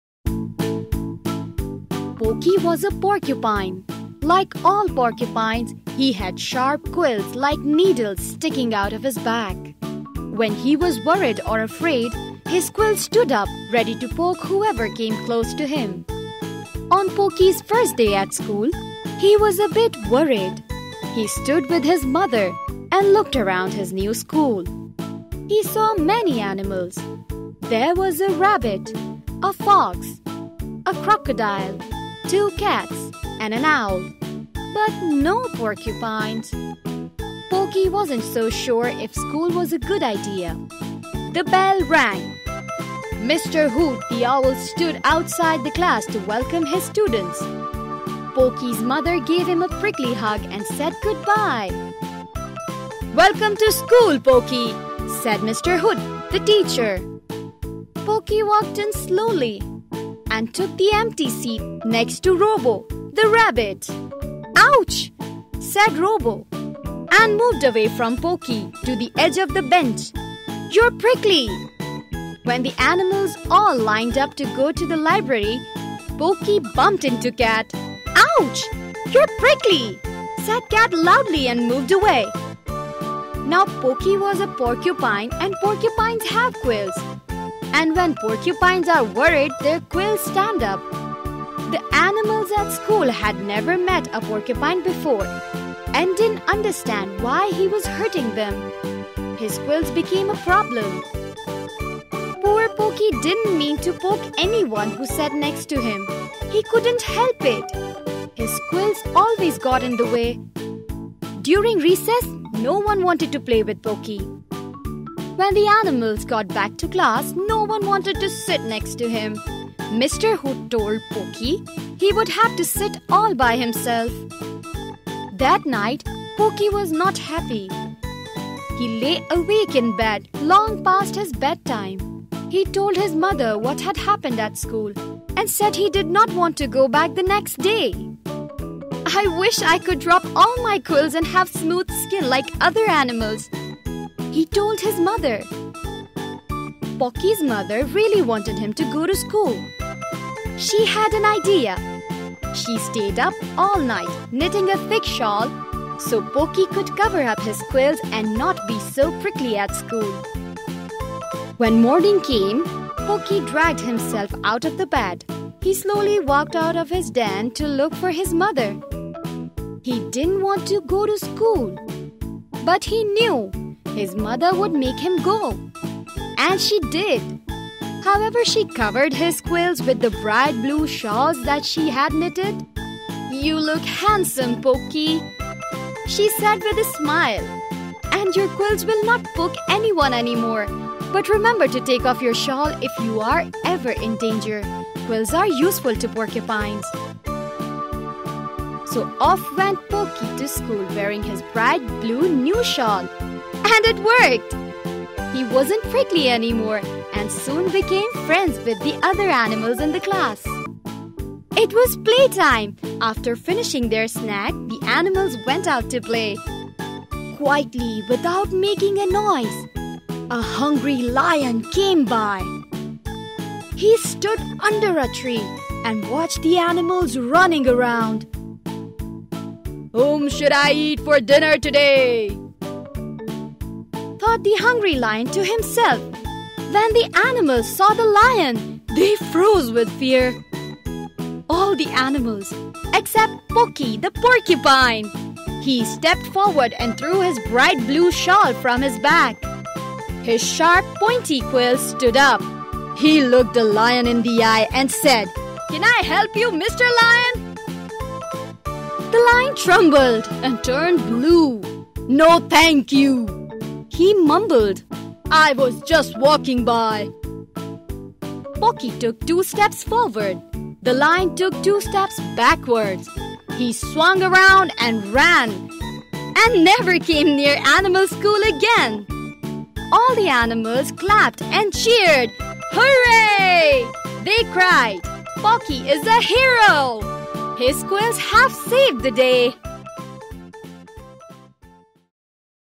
Pokey the Porcupine I English I Unit 2 Grade 3
This animated video for Grade 3 students describes a story which can be used to improve kids listening skills by helping them recognize the sequence of events within the story by summarizing what happened with accuracy.